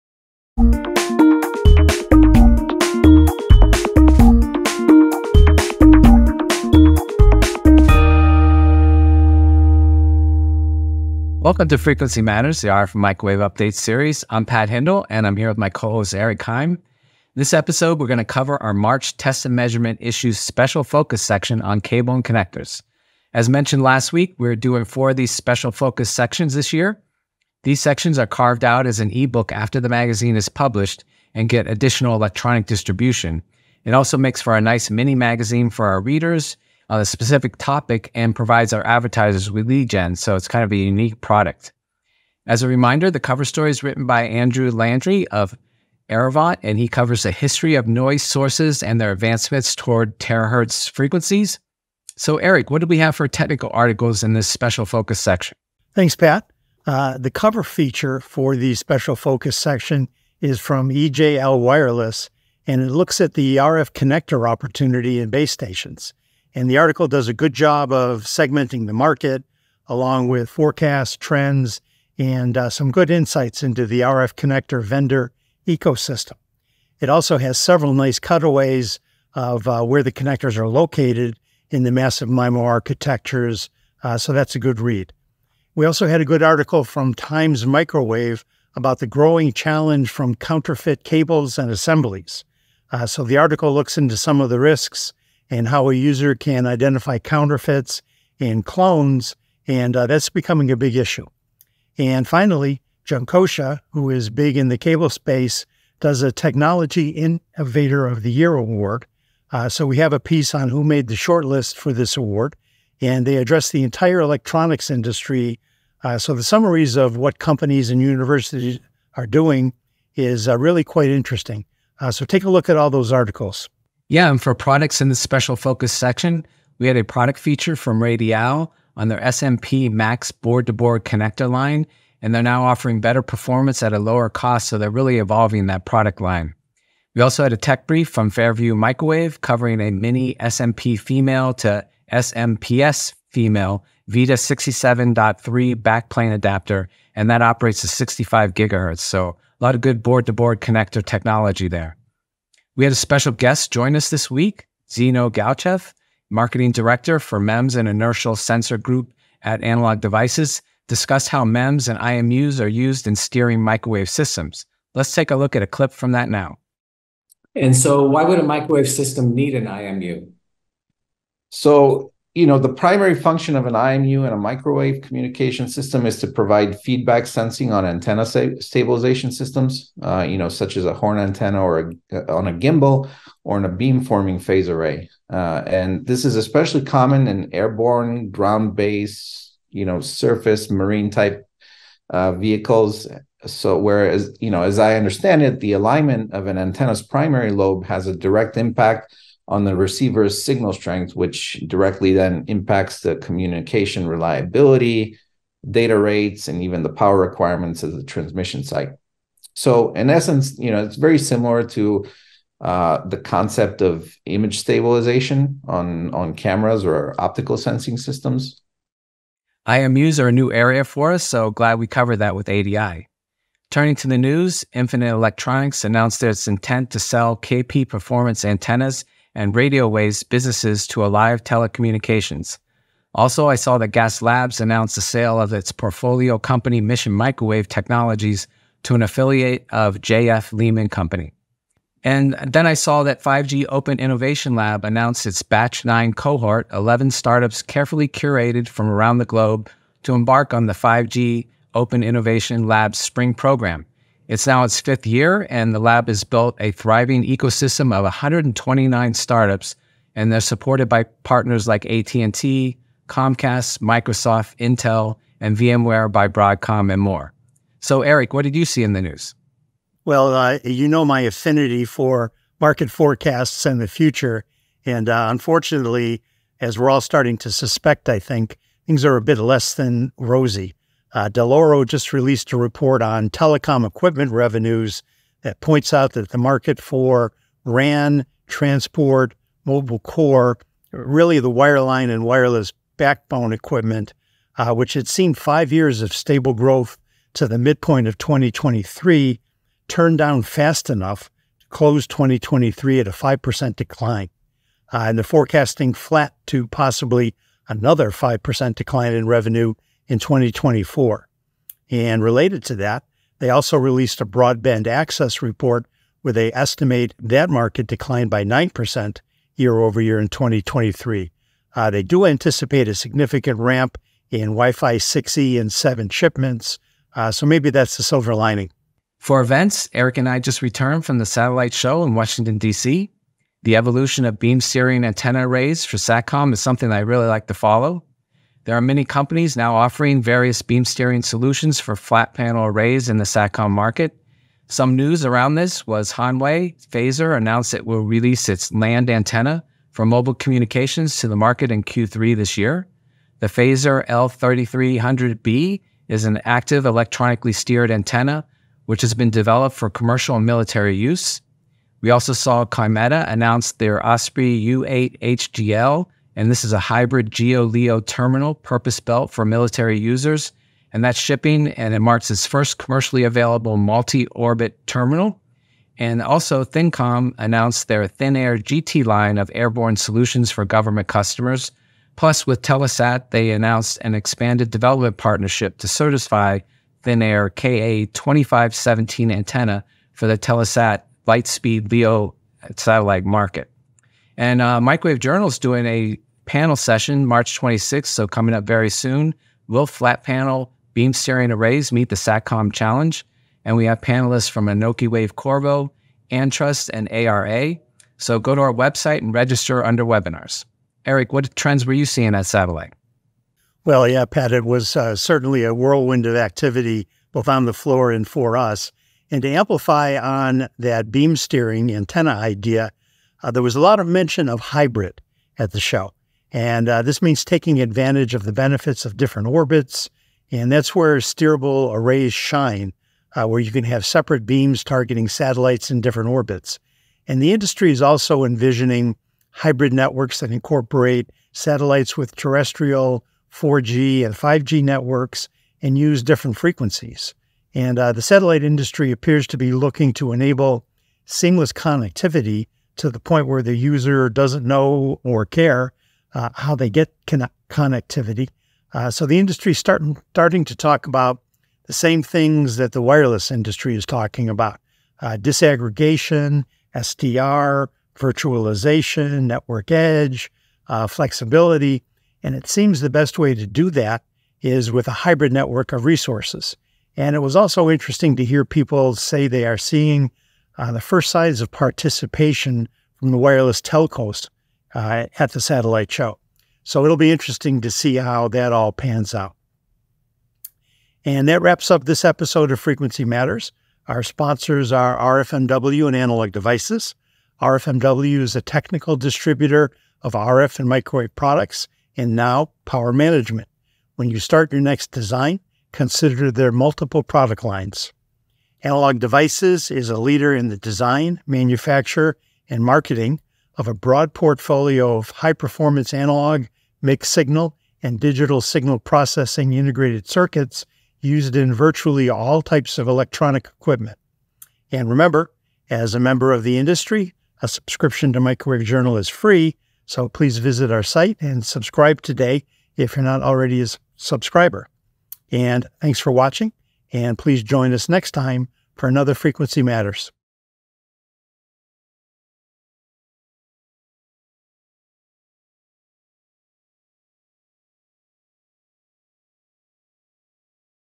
Frequency Matters, March 22: Cable/Connectors, ADI Interview, Industry News/Satellite 2024
Microwave Journal editors Pat Hindle and Eric Higham cover the March Cable and Connector special focus articles, interview ADI's Tzeno Galchev about IMUs for microwave applications, industry news and Satellite 2024 event.